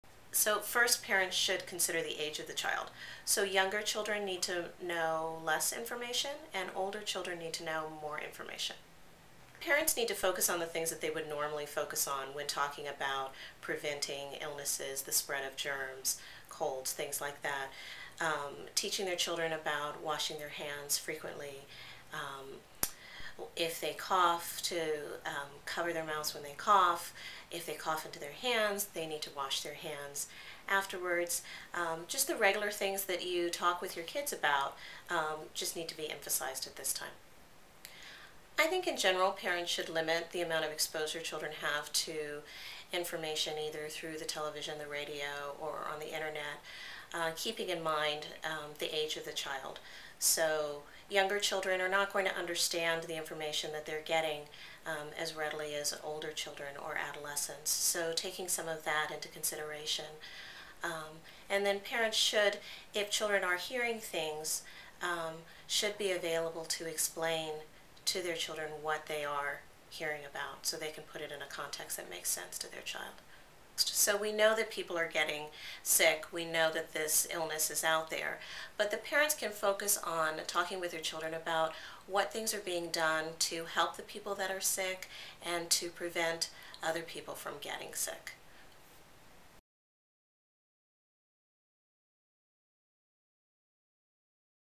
Talking to kids about swine flu, Packard Children's Hospital/Stanford
Sharon Williams, PhD, offers these tips for talking to your kids about the swine flu. Williams is director, outpatient services, and associate professor in the division of child and adolescent psychiatry at Lucile Packard Children's Hospital and the Stanford University School of Medicine.